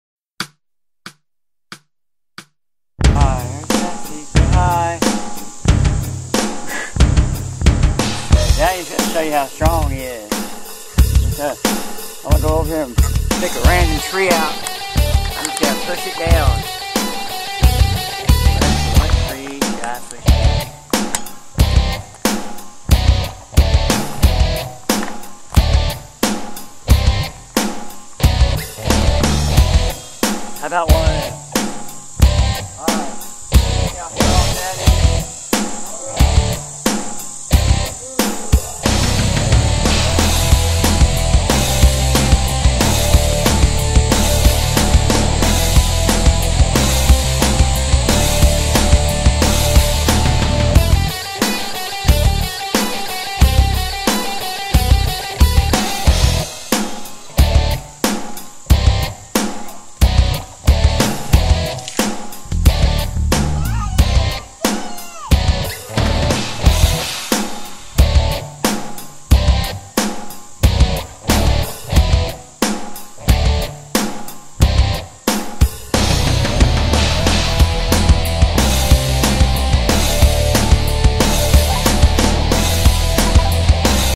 Daughter watches Superdad push over TREE!
My Daughter Jessica wanted to see how strong Daddy was... So one morning, I showed her by pushing over a tree in the backyard...She kept saying Daddy You Did It!!!